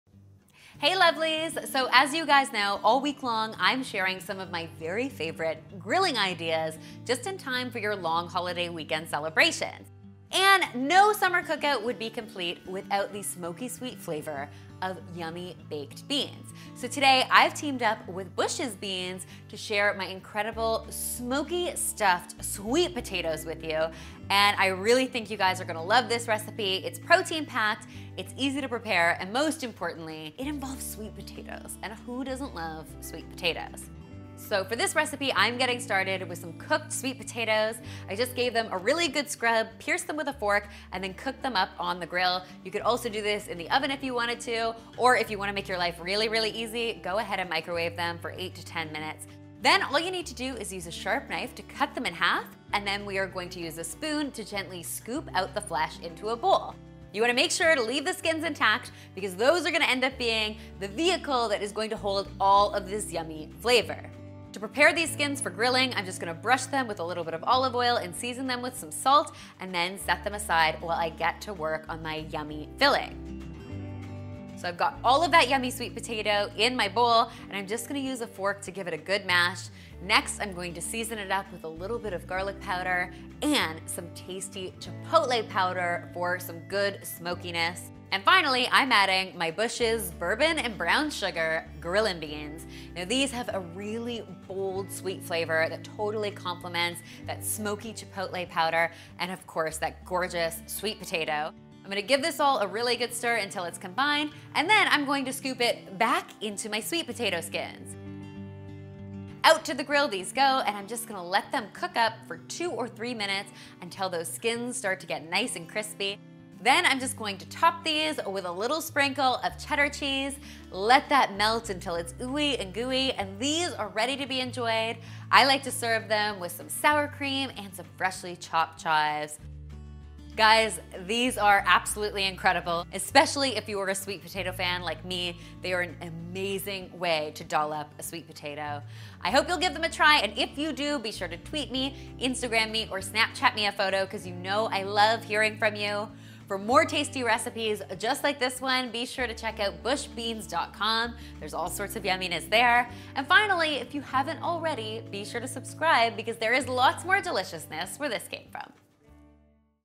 Smoky Stuffed Sweet Potato Recipe | Vegetarian BBQ
Recipe Below!
This video was made in collaboration with BUSH'S Beans.
For more recipes & inspiration visit: Bushbeans.com/

Today we're making smoky grilled sweet potatoes that are perfect for your next BBQ!

Visit my Healthy Meal Plans website to start Meal Planning Now

Smoky Sweet Potatoes

Prep Time: 15 minutes
Cook Time: 15 minutes
Total Time: 30 minutes
Serves: 4

2 large sweet potatoes
1 can BUSH’S Bourbon and Brown Sugar Grillin’ Beans
1⁄2 tsp garlic powder
1 tsp chipotle powder OR smoked paprika
1⁄2 cup cheddar cheese, shredded
1⁄2 cup sour cream
2 tbsp fresh chives, chopped

1. Pierce sweet potatoes with a fork and then microwave for 8-10 minutes or until they’re cooked through.
2. Slice sweet potatoes in half and gently scoop out the flesh, leaving the skins intact. Brush the skin with olive oil and set aside.
3. In a large bowl combine cooked sweet potato, BUSH’S Bourbon and Brown Sugar Grillin’ beans, garlic powder, chipotle powder, salt and pepper. Stir to combine.
4. Scoop mixture back into each sweet potato skin. Top with shredded cheddar cheese.
5. Grill for 5 minutes until the skin becomes crispy and the cheddar is melted. Serve topped with sour cream and chives.